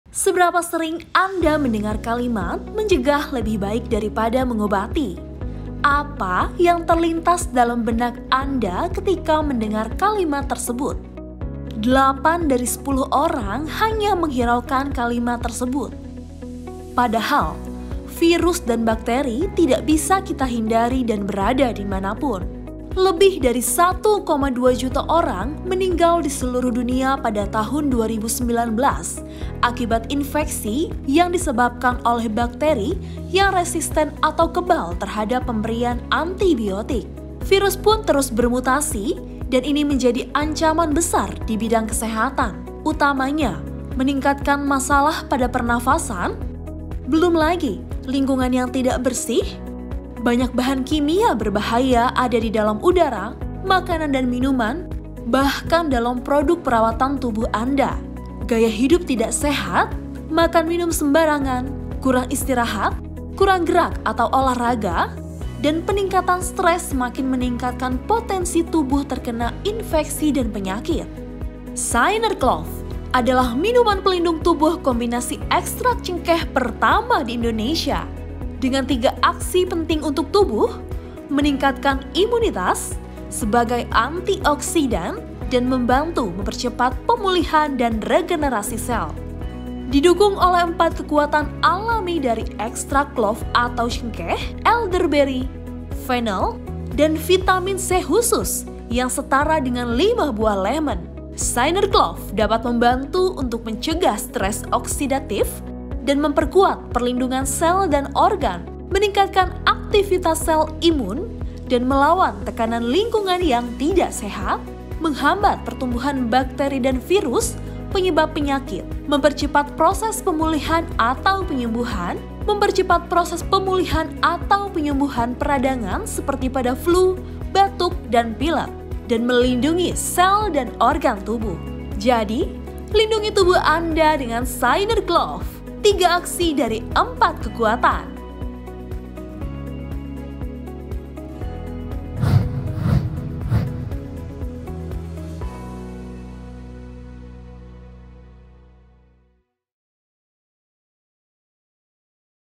SynerClove - Protection Premix
SynerClove adalah minuman herbal kombinasi ekstrak cengkeh pertama di Indonesia, sebagai pelindung tubuh yang berfungsi untuk meningkatkan imunitas dengan menghambat pertumbuhan dan melawan bakteri dan virus penyebab penyakit, sebagai antioksidan untuk memperkuat perlindungan sel dan organ, serta mempercepat regenerasi atau pemulihan dan penyembuhan dari infeksi seperti flu, batuk dan pilek dan peradangan atau nyeri pada organ tubuh lainnya